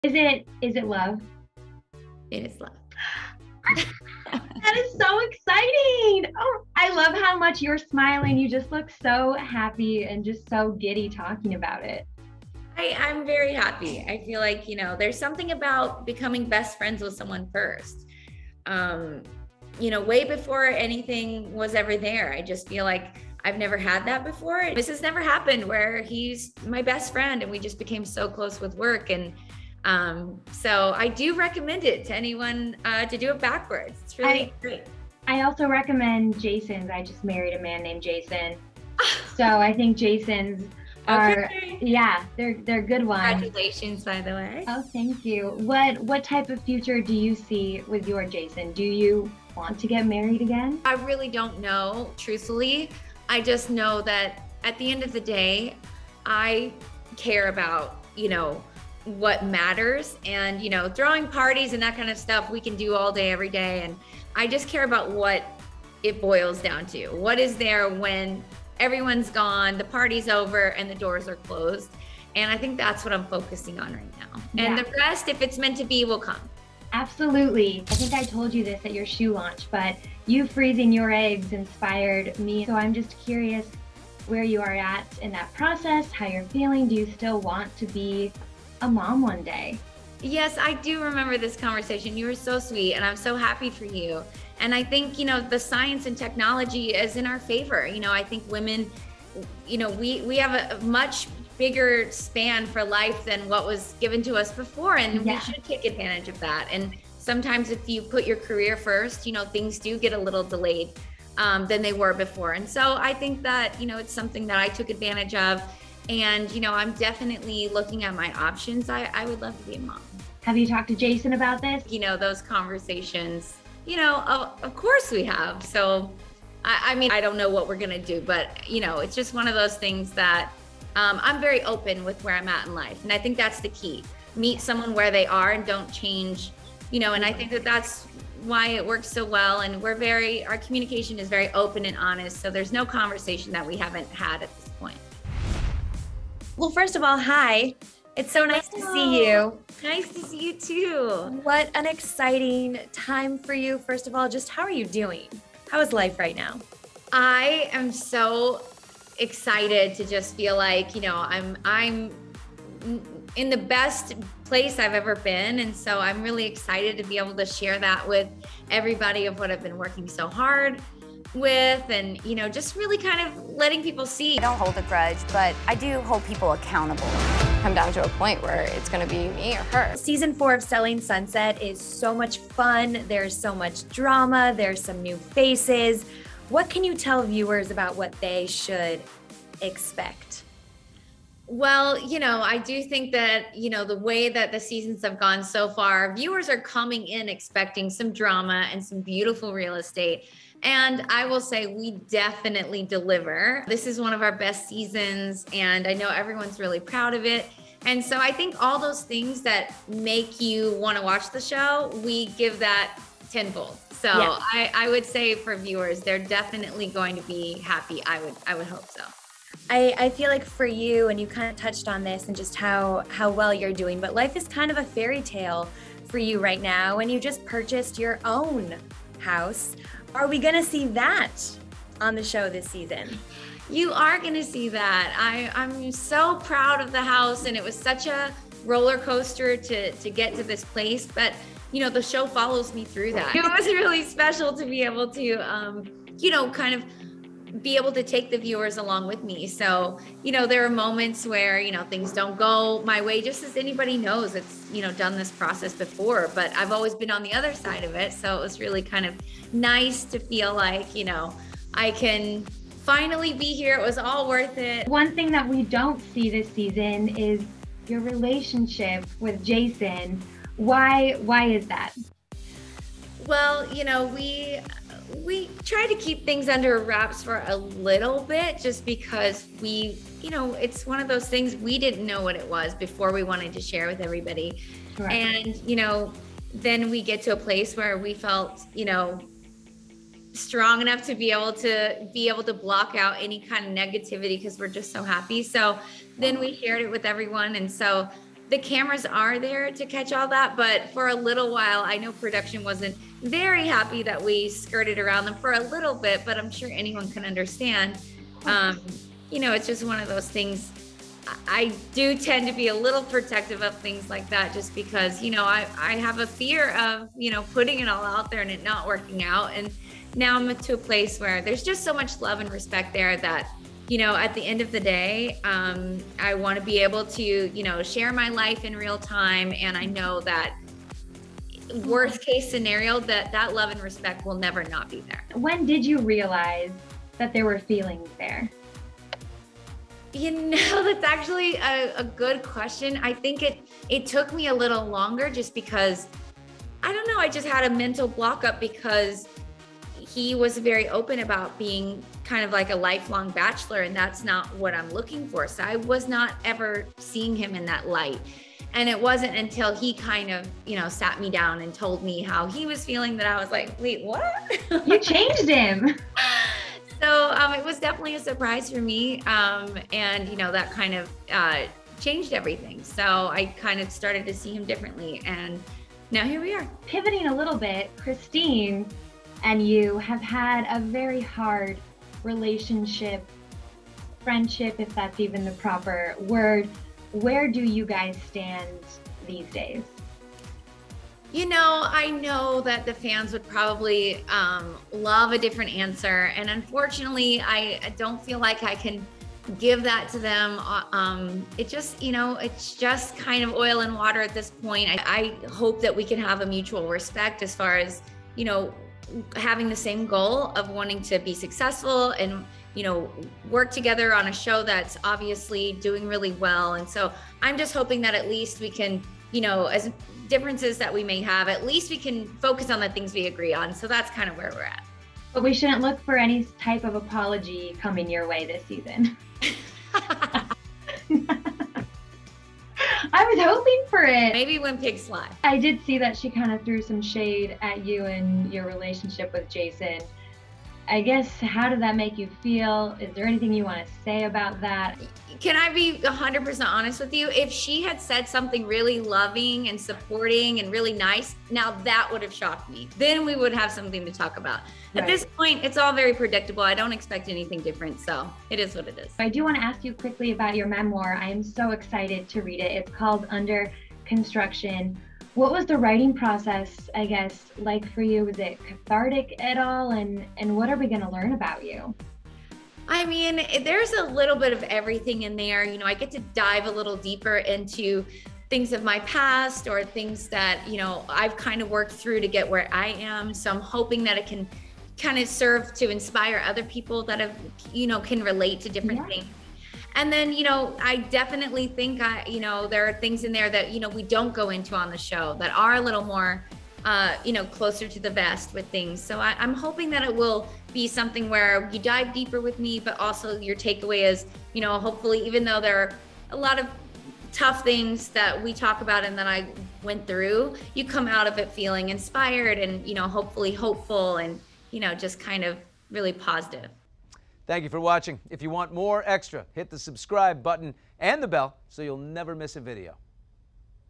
Chrishell Stause Opens Up About LOVE with Jason Oppenheim (Exclusive)
Chrishell Stause is getting candid with ‘Extra’s’ Katie Krause, revealing if she wants to get married again and if she and boyfriend Jason Oppenheim have discussed having children. The ‘Selling Sunset’ star also reveals when she and Jason realized there were feelings between them and admits it’s definitely ‘love.’ The reality star explains why their relationship won’t be shown on Season 4 of ‘Selling Sunset,’ which premieres November 24 on Netflix. The 40-year-old beauty also shares some insight into her upcoming memoir, ‘Under Construction,’ slated for release in 2022.